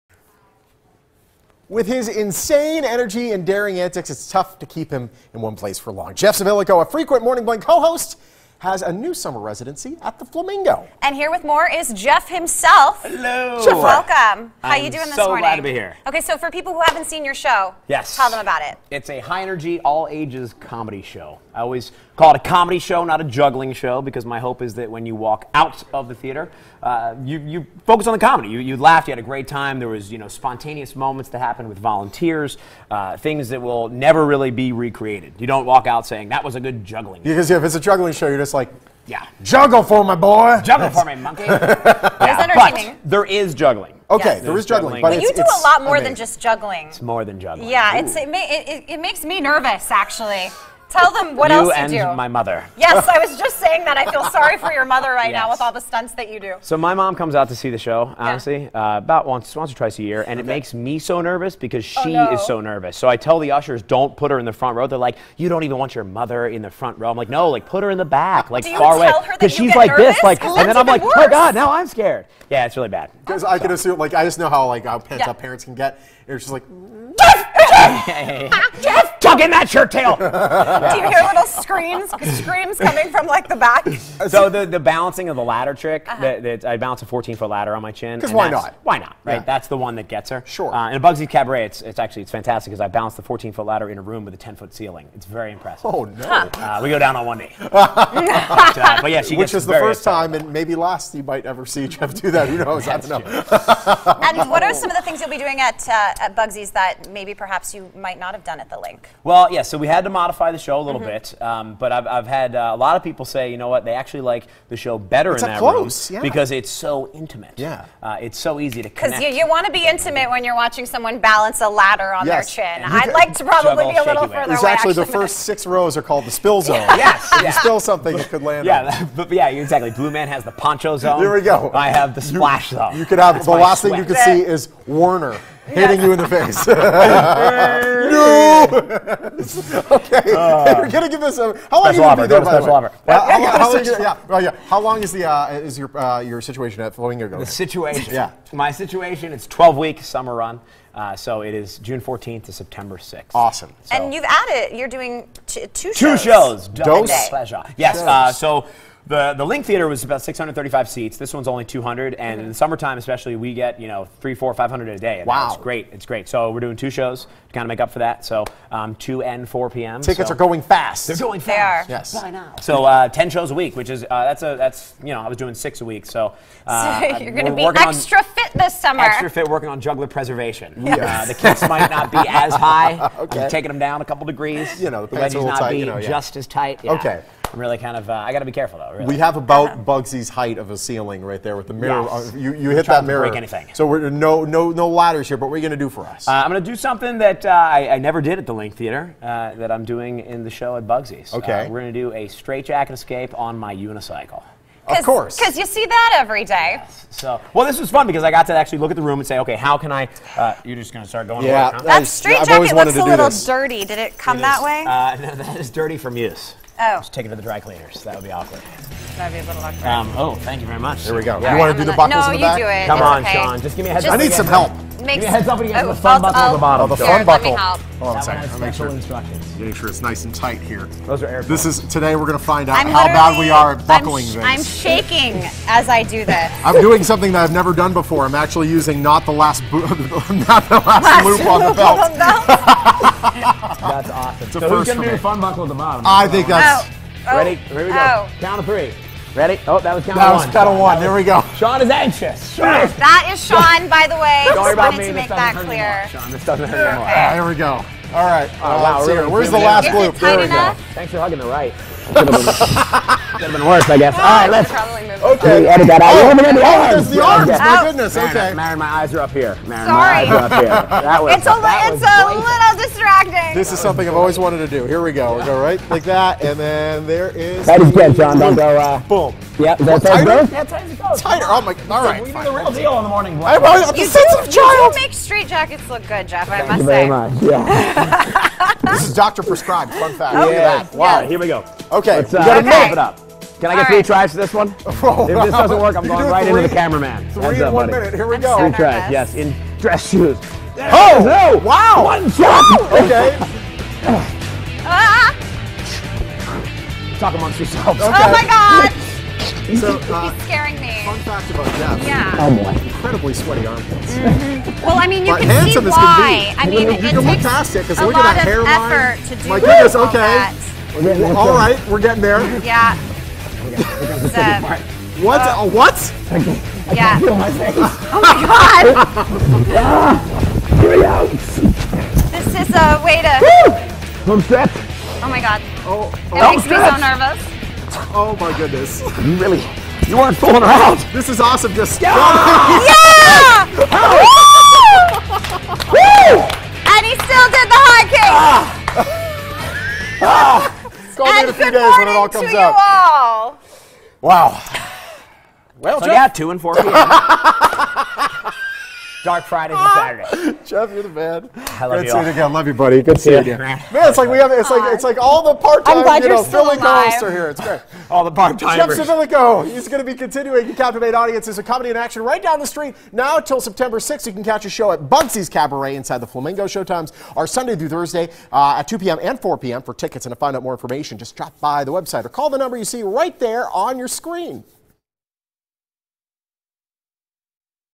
Laugh Until You Cry 6/26/15
Looking for a great family show this summer? Go see comedian, juggler and entertainer Jeff Civillico. He can balance a 12-foot ladder off his chin, juggle a bowling ball, apple and knife all the same time! Watch today to see crazy act he has in store for you!